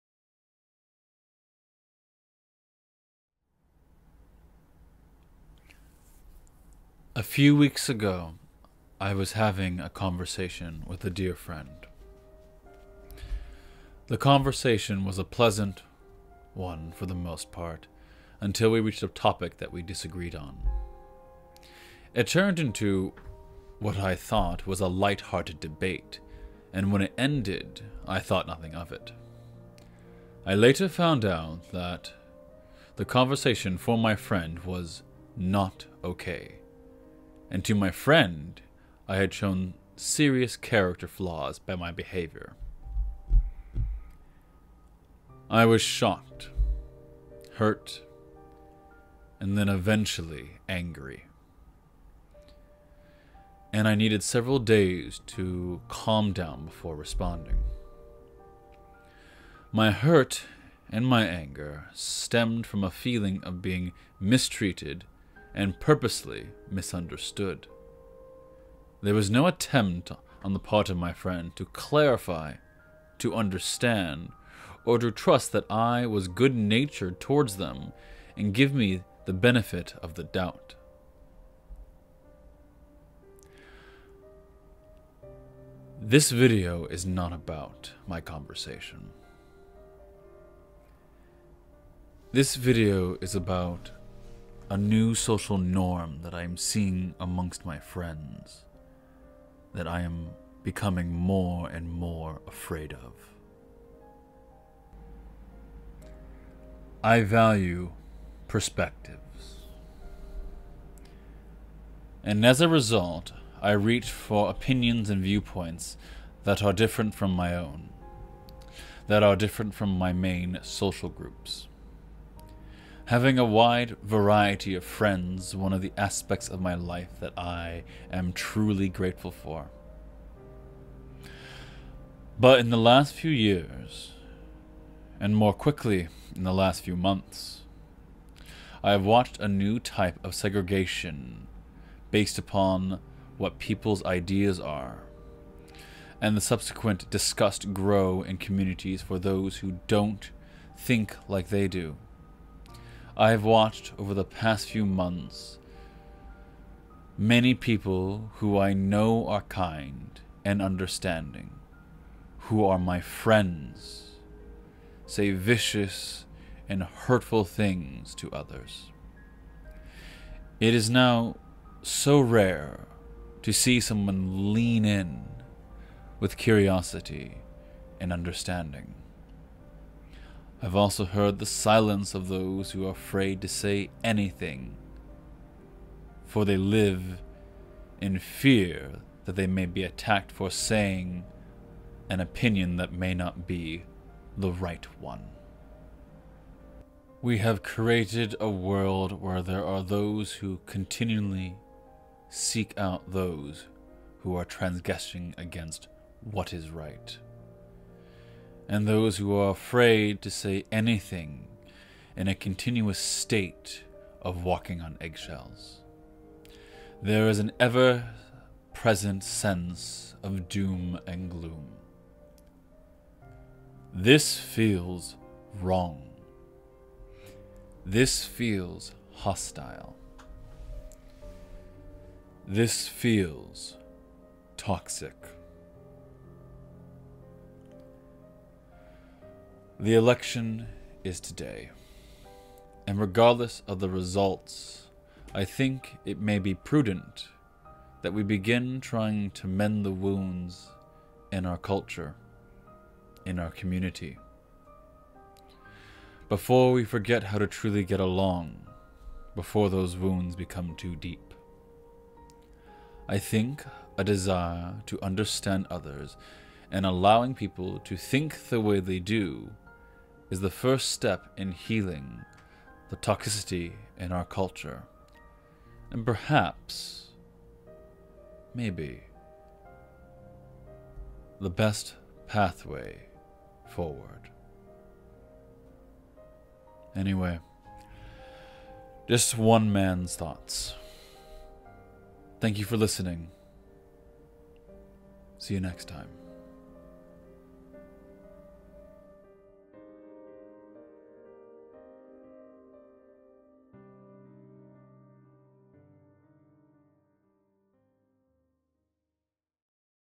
What has happened..
A few weeks ago I was having a conversation with a dear friend. The conversation was pleasant for the most part until we reached a topic that we disagreed on. It turned into what I thought was a light hearted debate and when it ended I thought nothing of it. I later found out that the conversation for my friend was not okay. And to my friend I had shown serious character flaws by my behavior.. I was shocked, hurt, then angry. And I needed several days to calm down before I responded. My hurt and my anger stemmed from a feeling of being mistreated and purposely misunderstood. There was no attempt on the part of my friend to clarify, to understand, or trust that I was good natured to them and give me the benefit of the doubt.
This video is not about my conversation. This video is about a new social norm that I am becoming more and more afraid of.

I value perspectives, and as a result I reach for opinions and viewpoints that are different from my own… that are different form my main social groups. Having a wide variety of friends is one of the aspects of my life that I am grateful for. But for the past few years, and even faster in the past few months, I have watched a new type of segregation based on what people’s ideas are, and the subsequent disgust grow in  communities for those who don’t think like they do. I have watched over these past few months many people who I know are kind and understanding say viscous and hurtful things to others through social media. It is rare to see someone lean in with curiosity and understand. I’ve also heard the silence of those who are afraid to say anything, for they fear they may be attacked for their opinion because it may not be the “right one”…

We have created a world where there are those continually seeking out transgressors against “what is right”, and those who are afraid of saying anything, in a continuous state of walking on eggshells. There is an ever present sense of doom and gloom.

This feels Wrong…
This feels Hostile…
This feels Toxic…

The election is in two days… and regardless of the results I think it is imperative that we begin to mend the wounds in our culture. Before we forget how to truly get along. I think that a desire to understand others and allowing people to think the way they do is the first step in healing the toxicity in our culture and perhaps to finding the best path forward.